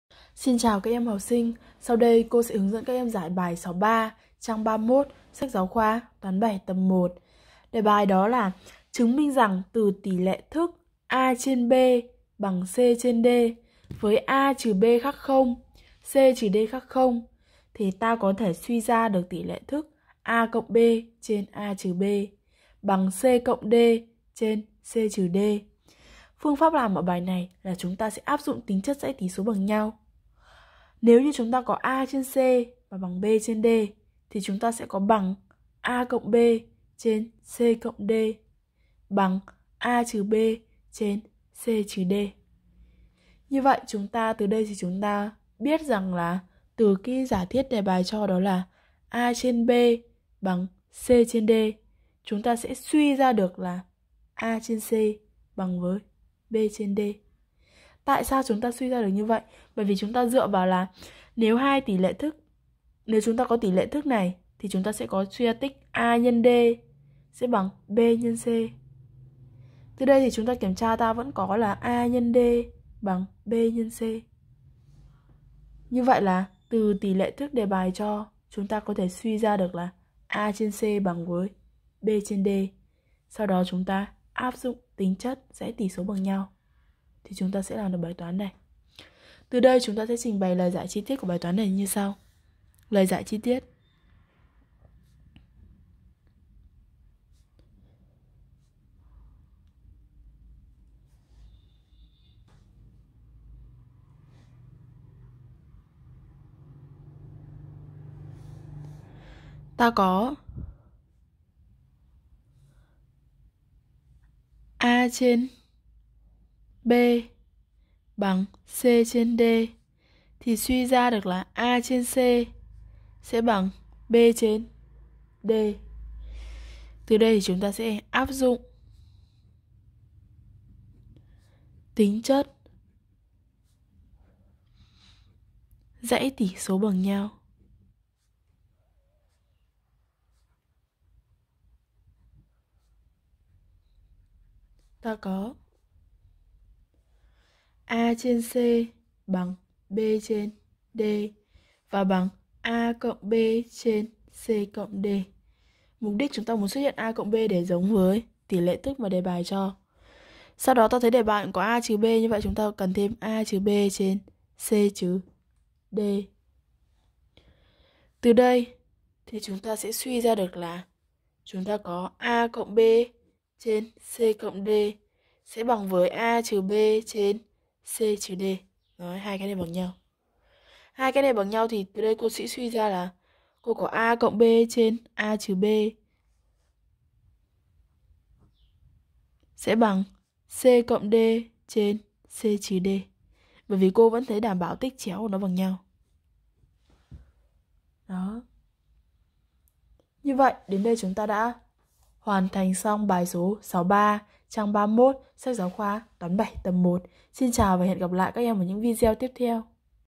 Giải bài 63 trang 31 SGK toán 7 tập 1
Chứng minh rằng từ tỉ lệ thức  \dfrac{a}{b} = \dfrac{c}{d}\,\,(a\, - b \ne 0,\,c\, - d \ne 0)  ta có thể suy ra tỉ lệ thức  \dfrac{{a + b}}{{a - b}} = \dfrac{{c + d}}{{c - d}} .